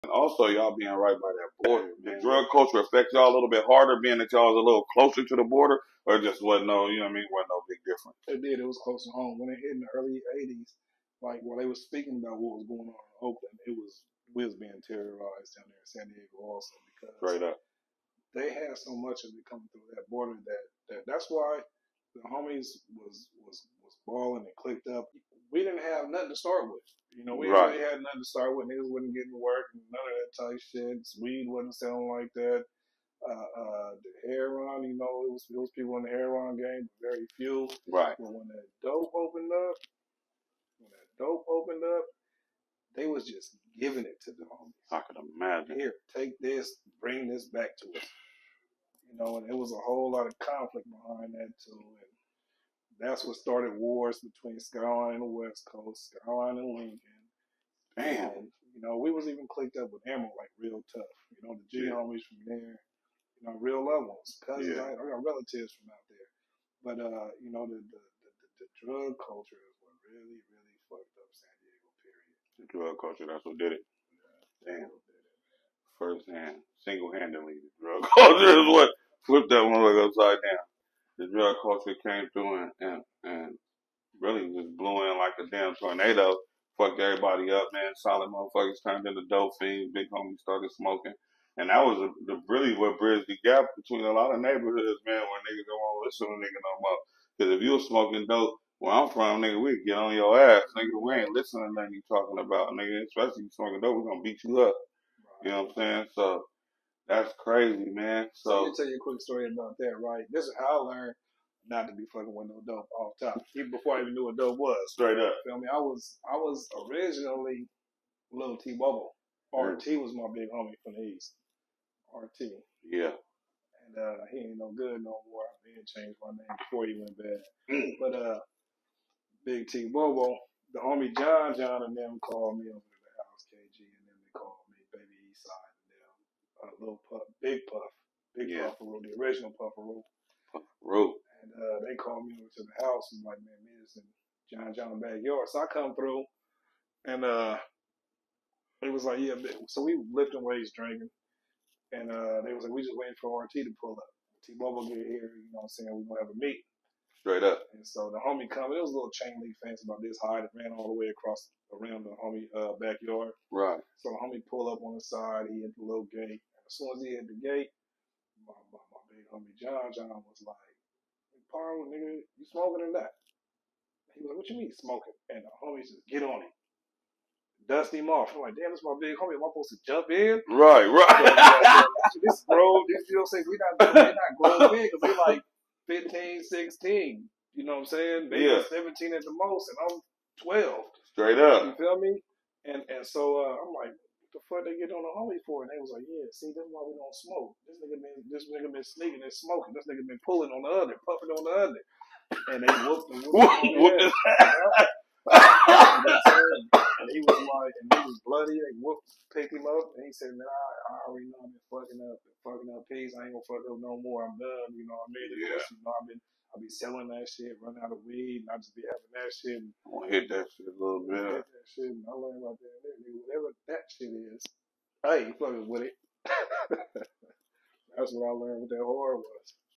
I survived 16 years in Ca Prison. San Diego’s Skyline Piru O.G. Big T Mighty Sits down with Us. Pt.3
We sit down with San Diego’s Skyline Piru O.G. Big T Mighty to discuss gang life, drug culture, prison life, cryptocurrency investing & much more. vlog #2023